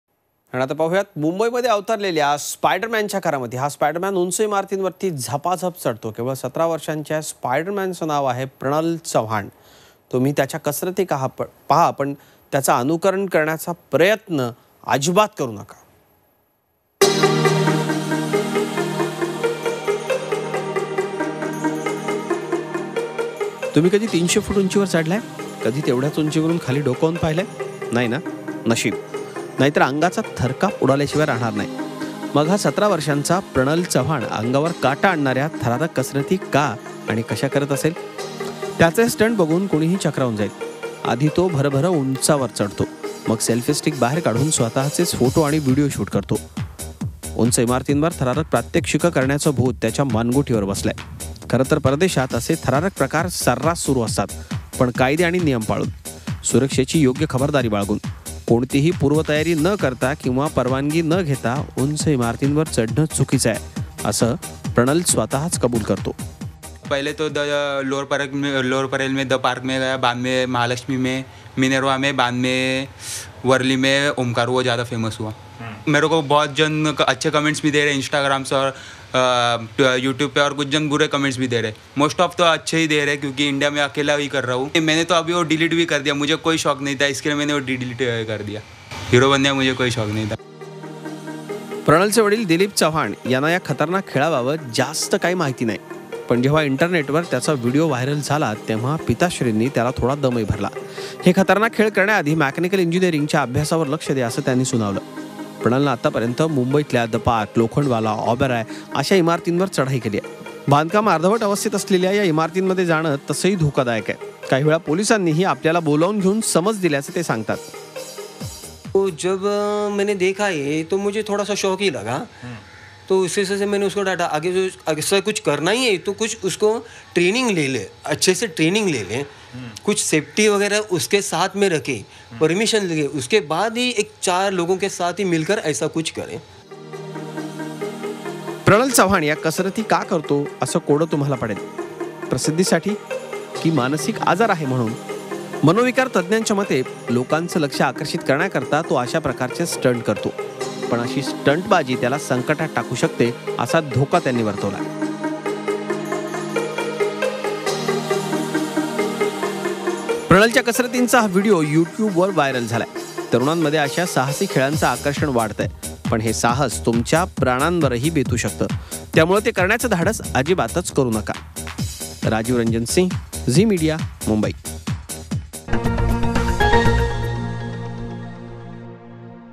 मुंबई । मेट्रो आयनॉक्सचं मुख्यमंत्र्यांच्या हस्ते उद्घाटन
Mumbai | Spiderman Climb On Tallest Building And Gets Selfie